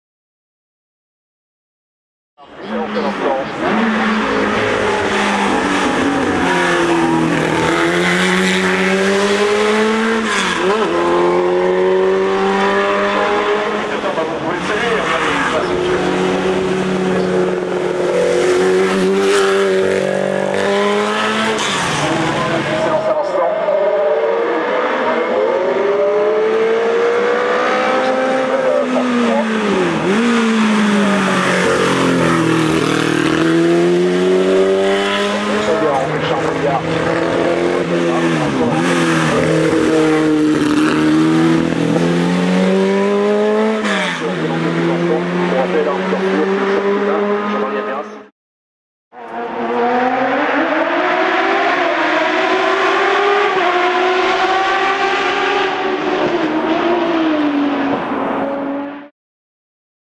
Martini MK15 - Viviane BONNARDEL - HILL CLIMB - 2023 - Turckheim-Trois Epis
Course de côte Turckheim - Trois-Épis